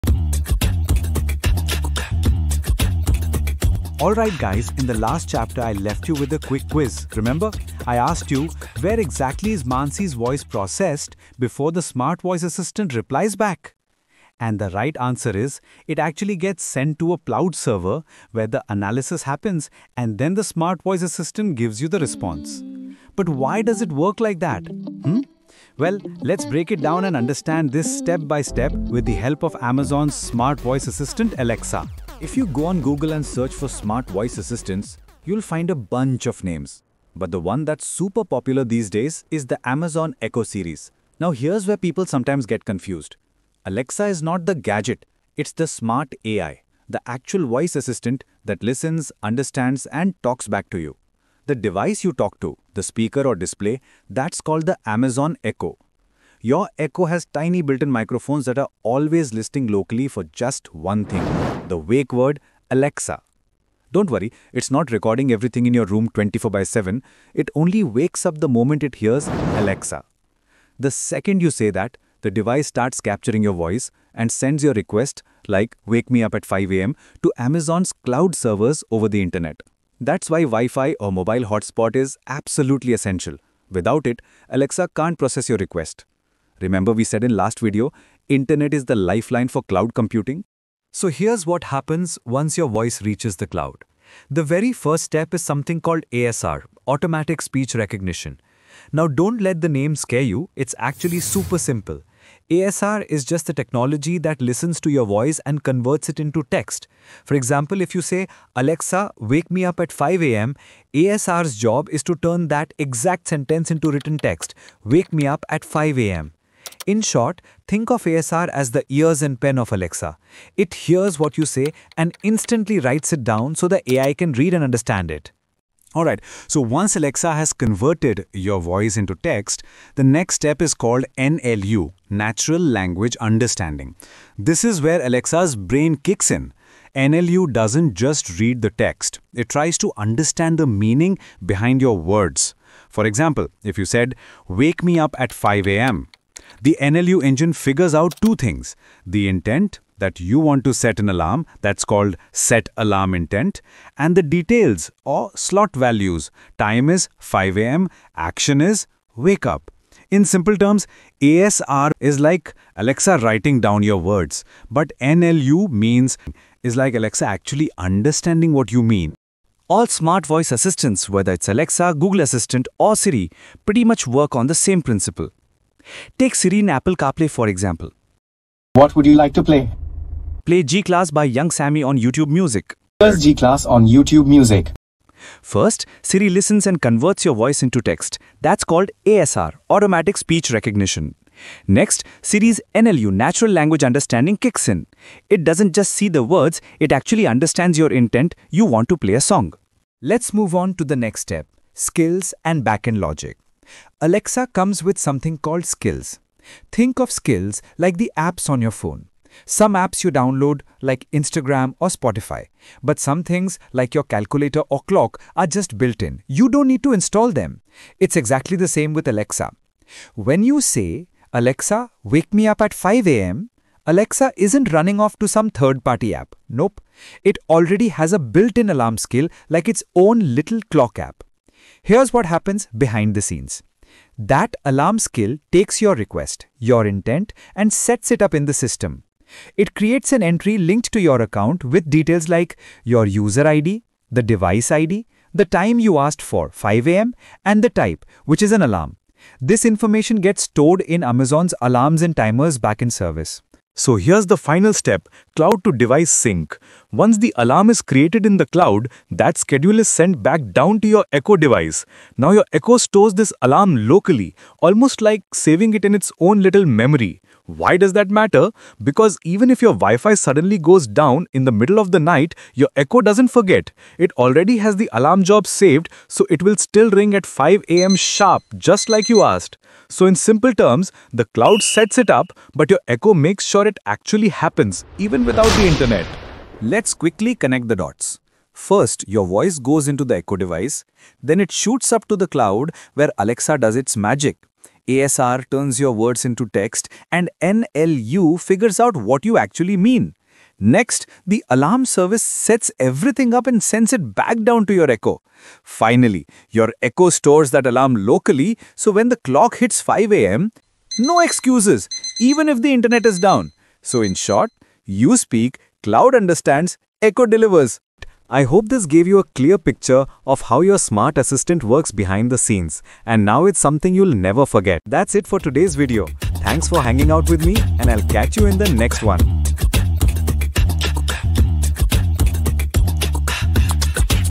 Amazon Echo
Understand how Alexa works behind the scenes, from voice input to cloud processing, using simple explanations of ASR, NLU, and backend services.
This beginner-friendly 5-minute explainer breaks down smart voice assistants like Alexa and Siri in an easy, real-world way.

📌 Use the timestamps below to navigate through the video with ease.

0:00 Intro
0:30 Smart Voice Assistant Quiz : Answer Reveal
0:53 Amazon Echo Series Smart Voice Assistant Alexa
1:18 Amazon Echo Listening to your command
1:55 ASR : Automatic Speech Recognition
2:29 NLU : Natural Language Understanding
3:11 Siri : In Apple Car Play
3:42 Alexa : Skills and Backend Logic | Amazon Alarm and Timer Backend Service
4:36 Alexa  : Cloud to Device Sync
5:10 How Alexa Works : Summary
5:47 Outro